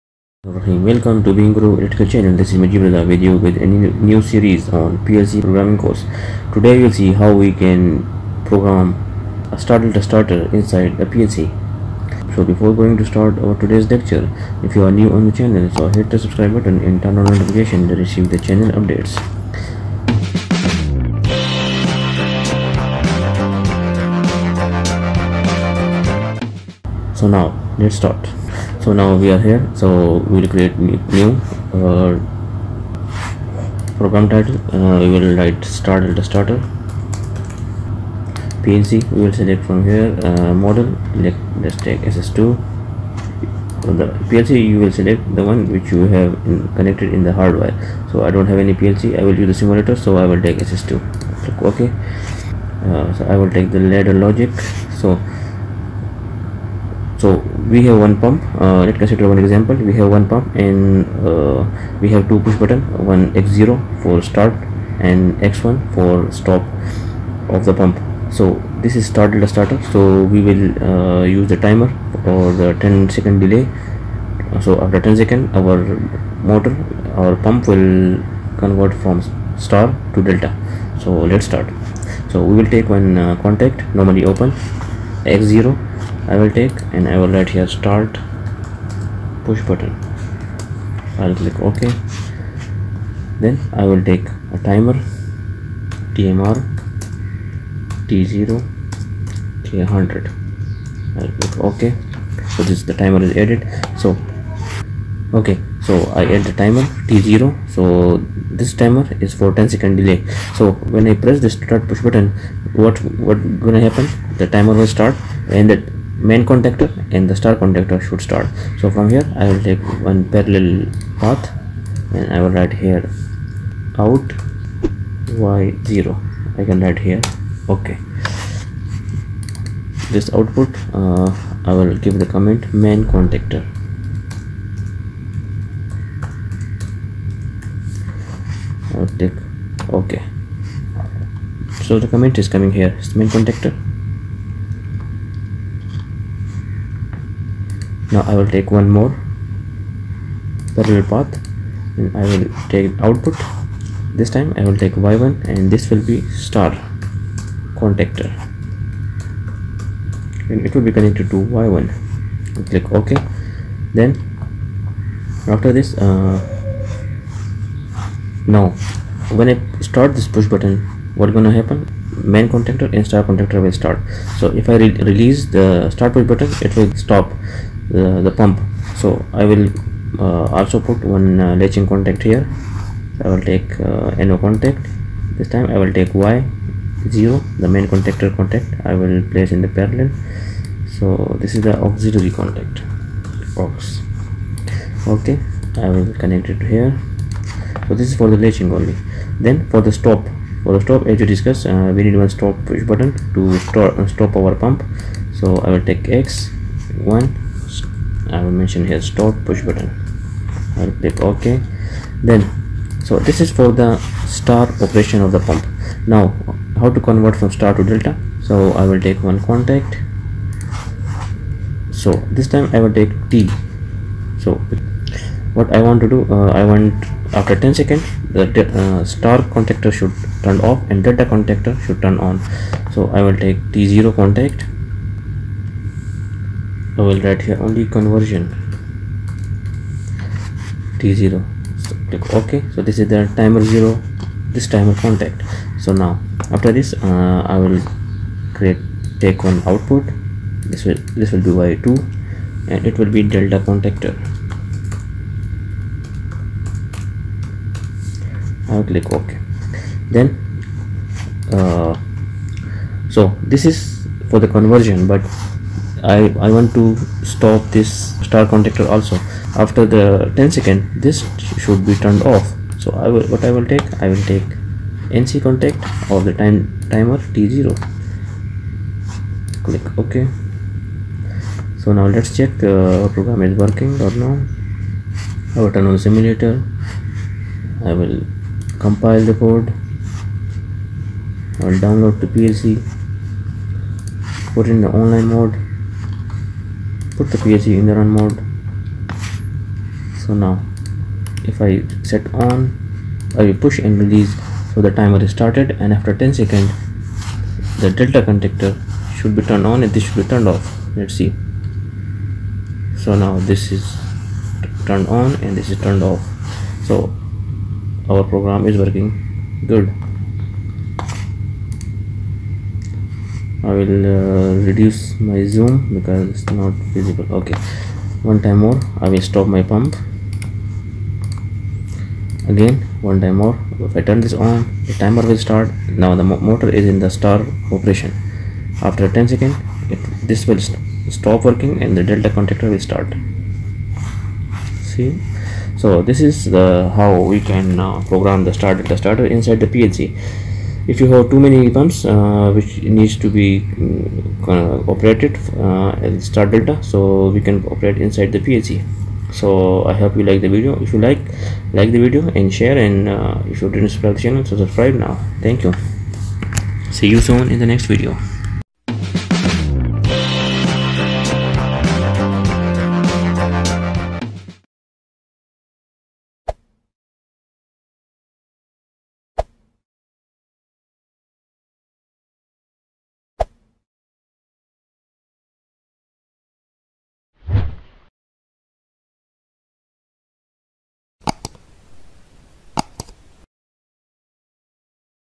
STAR DELTA STARTER IN DELTA PLC DVP-SS2 | Class 08 | PLC Ladder logic | BeingGuruElectrical
In this session 6 of the PLC programming free course, star-delta starter using on delay timers was demonstrated and programmed using WPL software,  this WPLSoft software is used to program delta programmable logic controllers, in this video timers, inputs, and outputs programming was discussed,

To understand this lecture you need to understand previous lectures in the PLC playlist. the link for which are attached below:

In case you have any comments put in the comments section down,

if you need a PowerPoint presentation in this class, put your email in a comment.